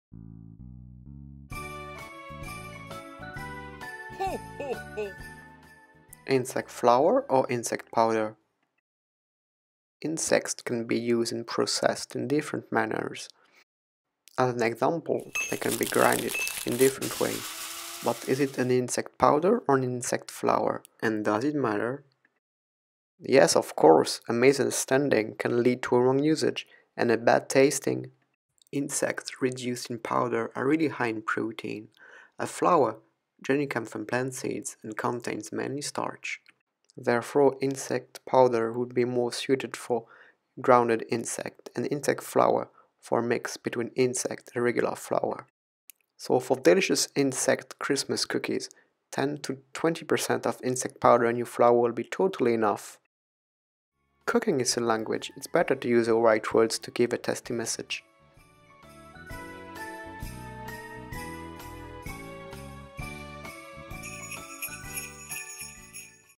INSECT POWDER OR INSECT FLOUR ? - Livin farms Advent calendar
Welcome to Livin farms Advent Calendar! Everyday until Christmas, we will share with you a delightful information about entomophagy (edible insects) food, nutrition or sustainability.

We often hear "Insect powder" and insect flour but is it the same?

Pre-order the first farm to grow edible insects in your kitchen and start a food revolution